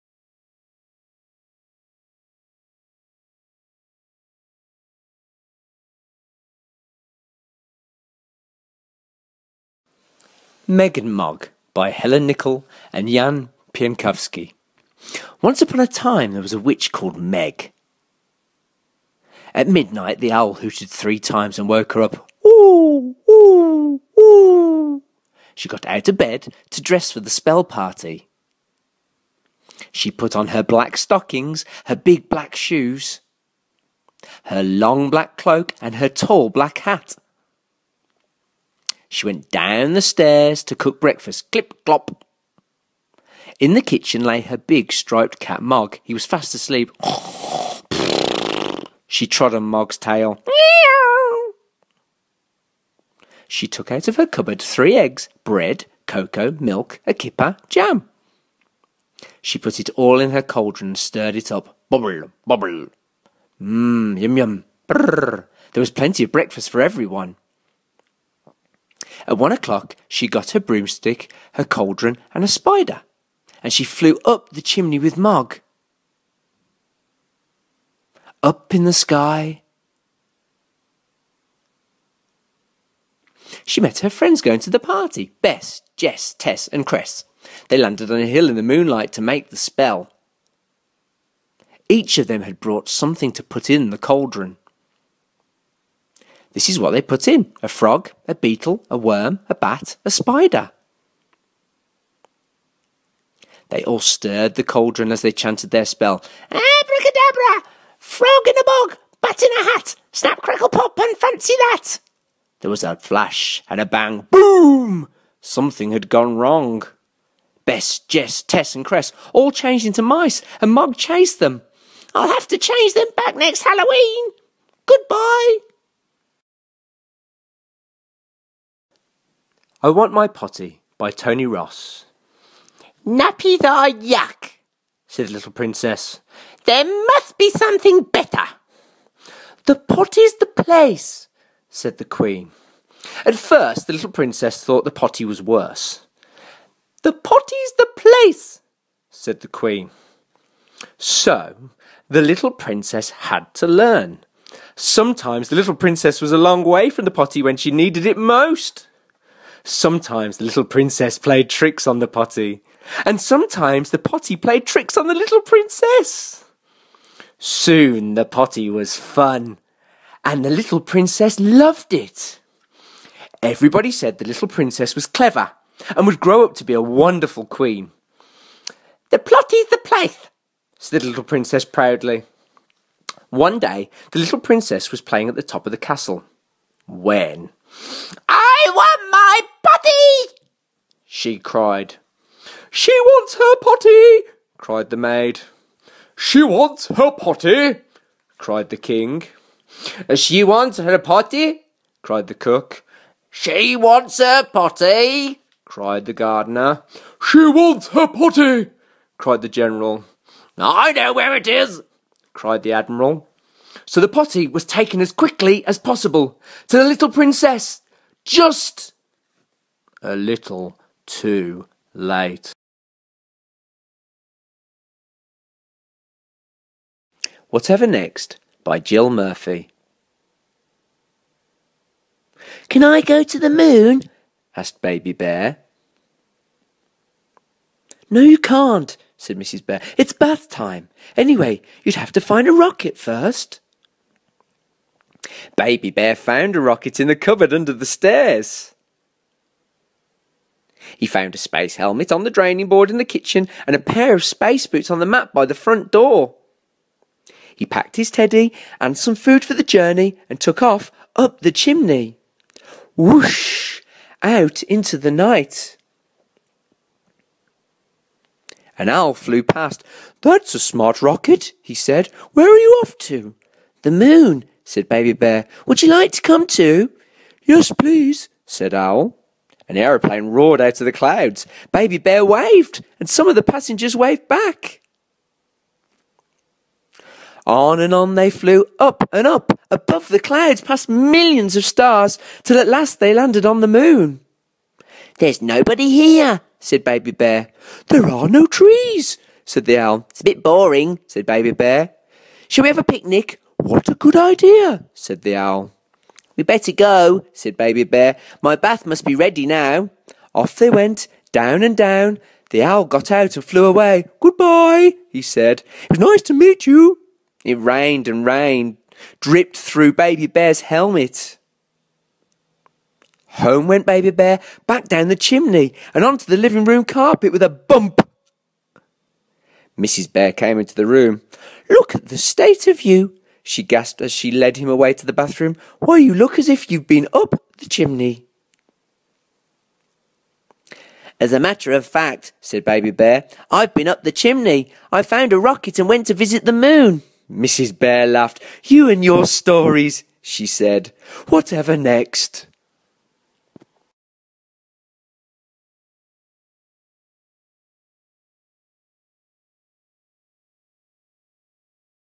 Bedtime stories
Stories before bed - Meg and Mog by Jan Pienkowski and Helen Nicoll, I Want My Potty by Tony Ross and Whatever Next! by Jill Murphy. Check out other bedtime stories on my Youtube Channel